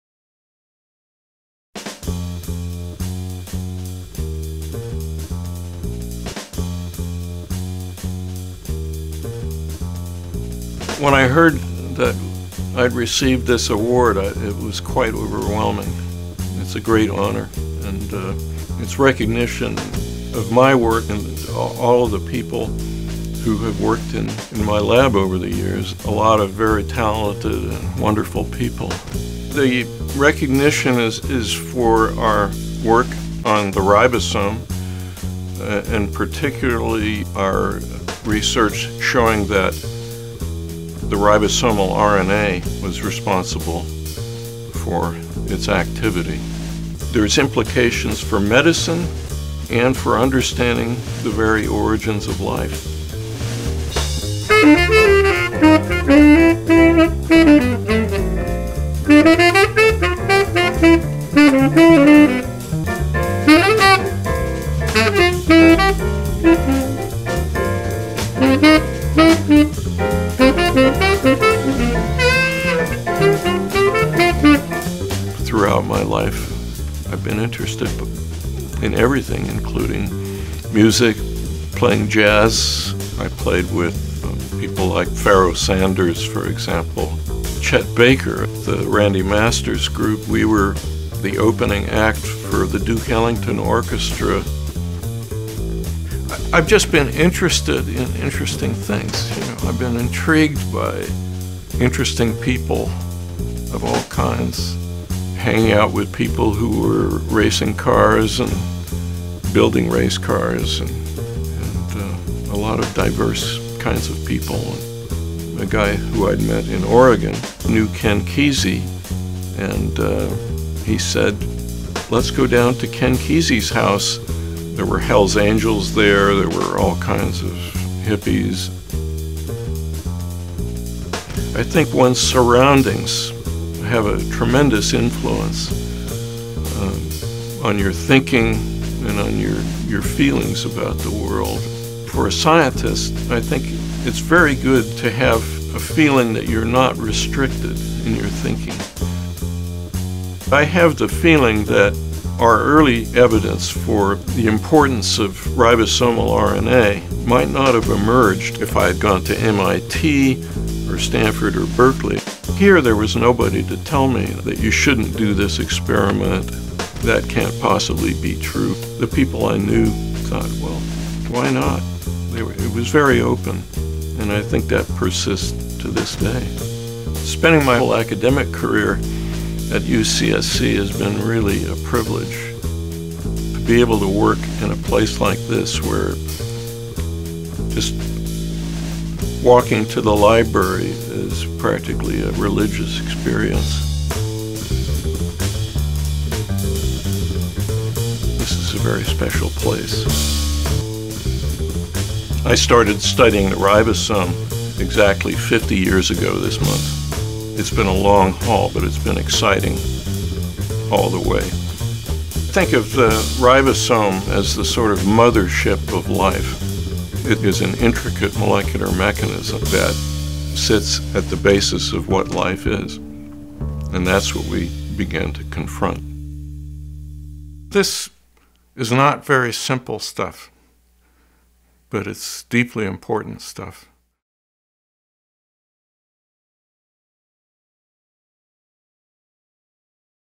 Challenging the Conventional - UC Santa Cruz Professor Harry Noller
A look at UC Santa Cruz biologist Harry Noller, "a tour de force in molecular biology for the past 50 years." Professor Noller has won the Breakthrough Prize in Life Sciences, worth $3 million. Noller’s research group was the first to solve the complete structure of a ribosome which led to shedding light on fundamental questions about the origins of life as a whole.

The Breakthrough Prize in Life Sciences honors transformative advances toward understanding living systems and extending human life, and was founded in 2013 by Silicon Valley tech entrepreneurs including Mark Zuckerberg, Anne Wojcicki, and Yuri Milner.